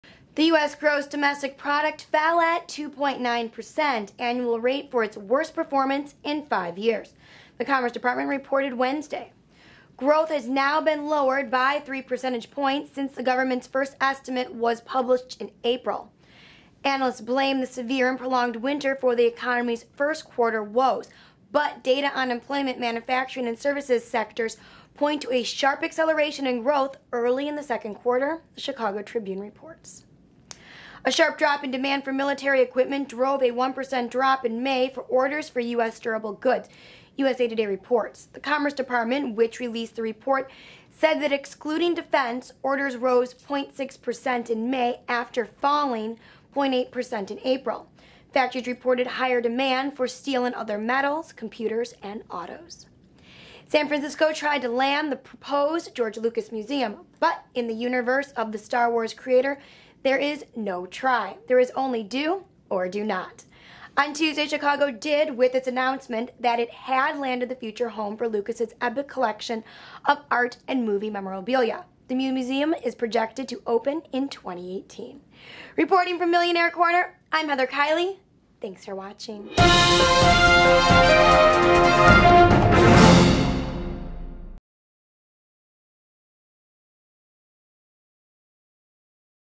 Star Wars Museum, US GDP, Durable Goods Manufacturing - Today's Financial News
Economy's first quarter woes, the Fed's forecast for a rise in interest rates and Walgreen's possible prescription to boost its bottom line top the day's roundup of top business stories.